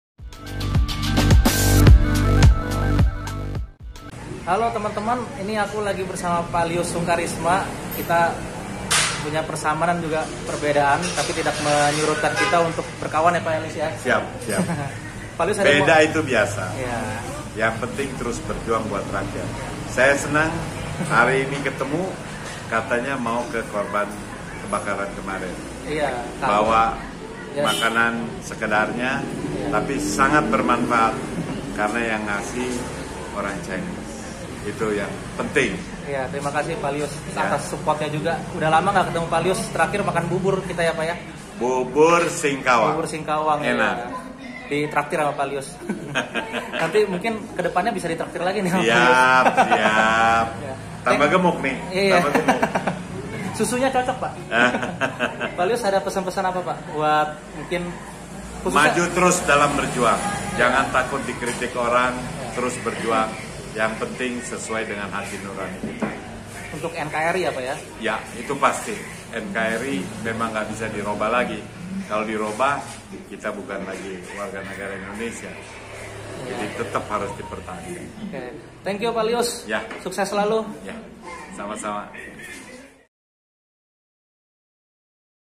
Advent Setiawan & Lieus Sungkharisma : 'Berbeda itu biasa,Bukan berarti harus bermusuhan'
Vlog : Advent Setiawan & Lieus Sungkharisma  'Berbeda itu biasa,Bukan berarti harus bermusuhan'

Meski kita 'tak sama bukan berarti kita harus bermusuhan,
Meski kita berbeda bukan berarti kita 'tak bisa bersahabat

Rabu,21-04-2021
Pukul:19.00wib
Kelurahan Keagungan/Kecamatan Taman Sari - Jakarta Barat

Video by KAMI RAYA (Komunitas Anak Muda Indonesia Raya)
Video Editor by Anyitko

Document of Advent Setiawan's Mega Data Library/2021
AdventSetiawanProduction 2021